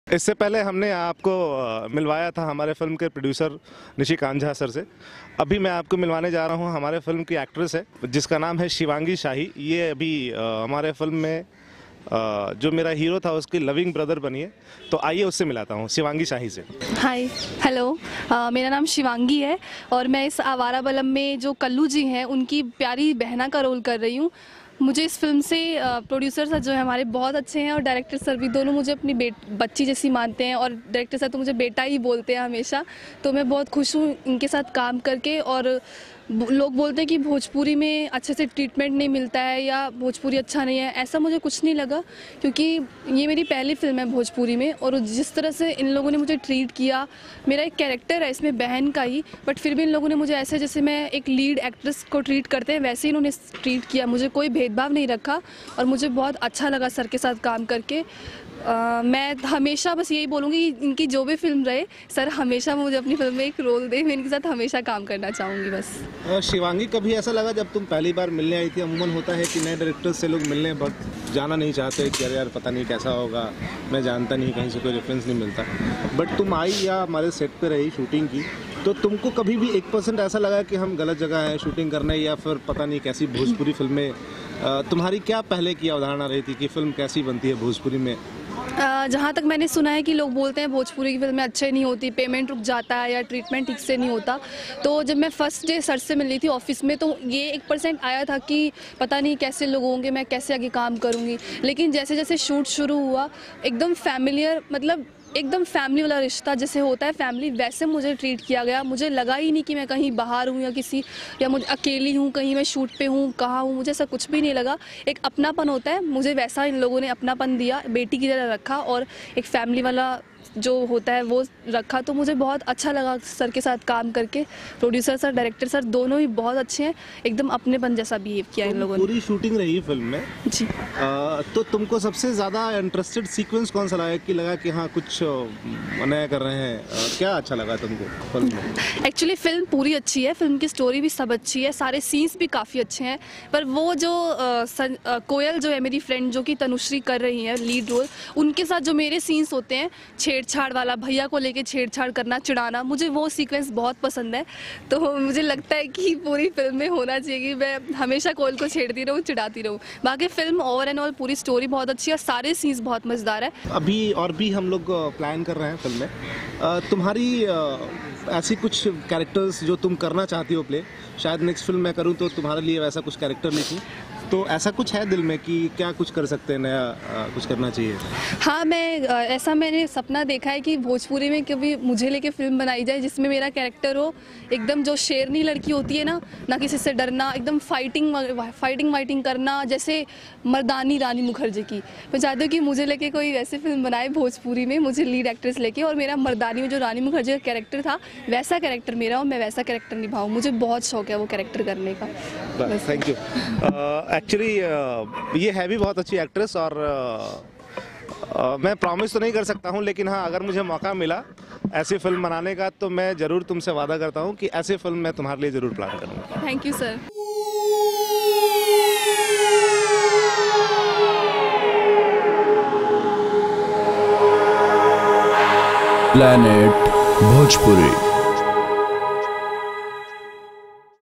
चन्दन उपाध्याय और शिवांगी शाही ने क्या बताया अपनी आने वाली फिल्म " आवारा बालम " के बारे में ?
Planet Bhojpuri is a destination for Bhojpuri Infotainment. चन्दन उपाध्याय और शिवांगी शाही !